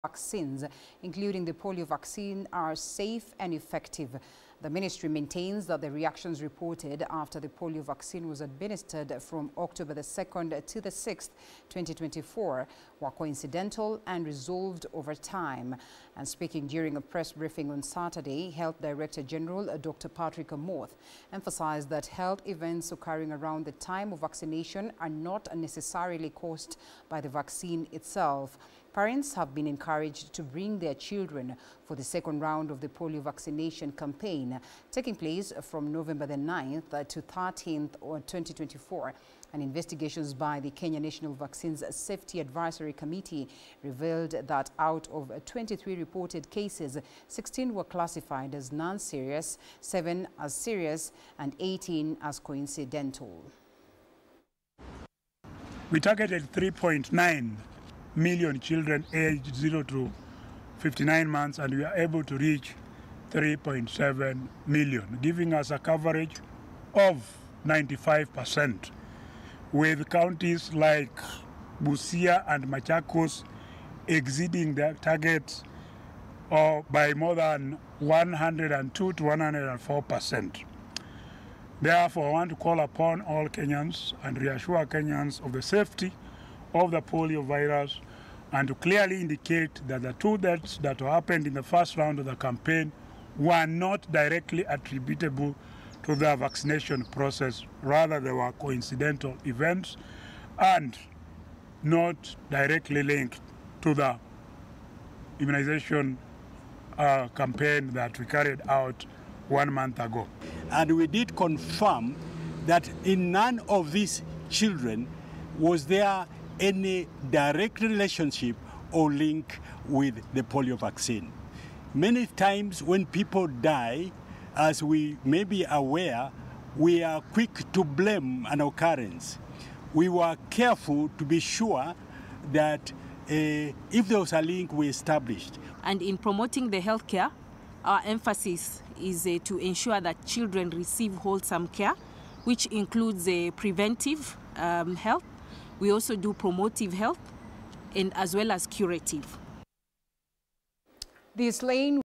The Ministry of Health assures the public, the Polio vaccine is safe and effective
MOH insists all vaccines including polio are safe and effective

MOH says the reactions from Polio vaccine were coincidental and resolved overtime

Parents urged to avail their children for the ongoing polio vaccination campaign

The campaign will take place from 9th to 14th 2024